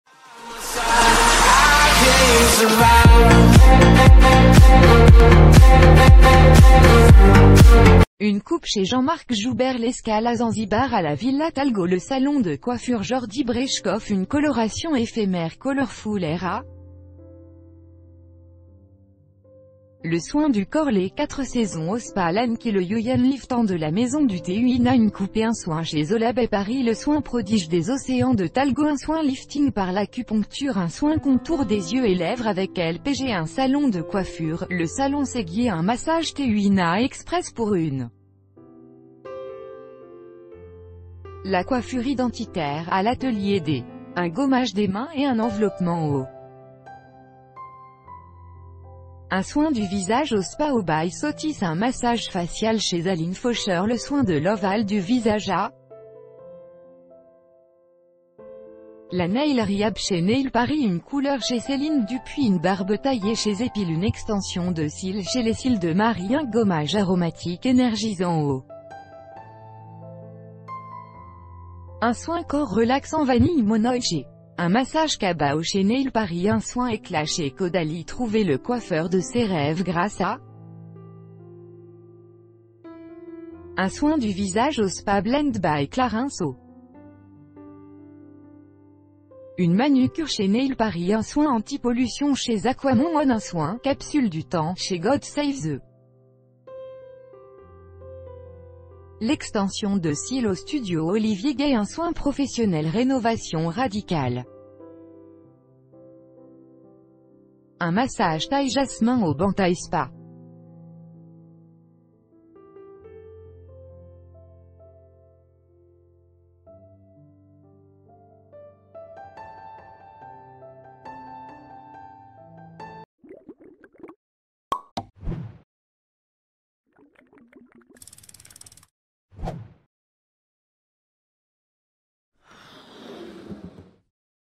Beauty Spot: nos adresses préférées | Végétarien
Une coupe chez Jean-Marc Joubert L’escale à Zanzibar à la Villa Thalgo Le salon de coiffure Jordy Brechkoff Une coloration éphémère Colorfulhair à... Le soin du corps Les 4 saisons au Spa Lanqi Le Yu Yan liftant de la Maison du Tui Na Une coupe et un soin chez OlabParis Le soin Prodige des océans de Thalgo Un soin lifting par l’acupuncture Un soin contour des yeux et lèvres avec LPG Un salon de coiffure : le salon Séguier Un massage Tui Na express pour une... La "coiffure identitaire" à l'Atelier des... Un gommage des mains et un enveloppement au... Un soin du visage au Spa O by Sothys Un massage facial chez Aline Faucheur Le soin de l’ovale du visage à... La nail rehab chez NailsParis Une c...